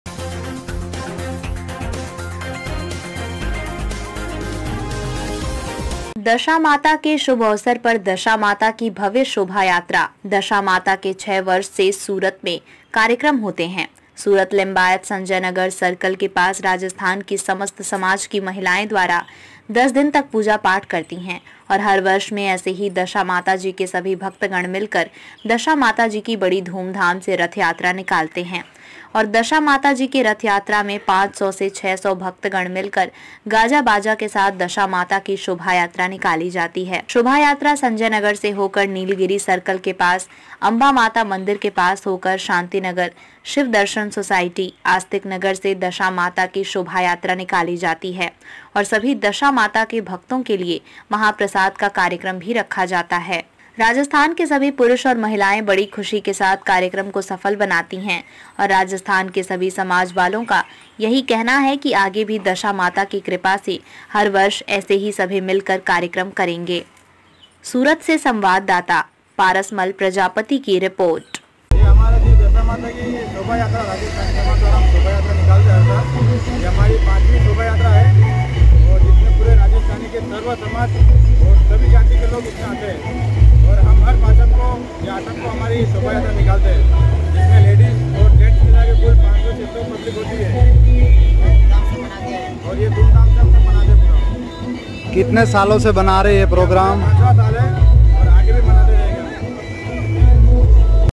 सूरत : दशा माता जी की शोभायात्रा का भव्य आयोजन..N.P-24NEWS TV..
दशा माता के शुभ अवसर पर दशा माता की भव्य शोभायात्रा दशा माता के 6 वर्ष से सूरत में कार्यक्रम होते है सूरत लिंबायत संजय नगर सर्कल के पास
राजस्थान की समस्त समाज की महिलाएं द्वारा
10 दिन तक पूजा पाठ करती हैं और हर वर्ष में
ऐसे ही दशा माता जी के सभी भक्त गण मिलकर
दशा माताजी की बड़ी धूमधाम से रथ यात्रा निकालते है और दशा माता जी के रथ यात्रा में 500 से 600 भक्त गण मिलकर गाजा बाजा के साथ दशा माता की शोभायात्रा निकाली जाती हैं शोभायात्रा संजय नगर से होकर नीलगिरी सर्कल के पास अंबा माता मंदिर के पास होकर शांति नगर, शिवदर्शन सोसाइटी, आस्तिक नगर, से दशा माता की सोभायात्रा निकाली जाती है और सभी दशा माता के भक्तो के लिए महाप्रसाद का
कार्यक्रम भी रखा जाता है राजस्थान के सभी पुरूष और महिलाए बड़ी खुशी के साथ कार्यक्रम को सफल बनाते हैं और राजस्थान के सभी समाज वालों का यही कहना है आगे भी दशा माता की कृपा से हर वर्ष ऐसे ही सभी मिलकर कार्यक्रम करेंगे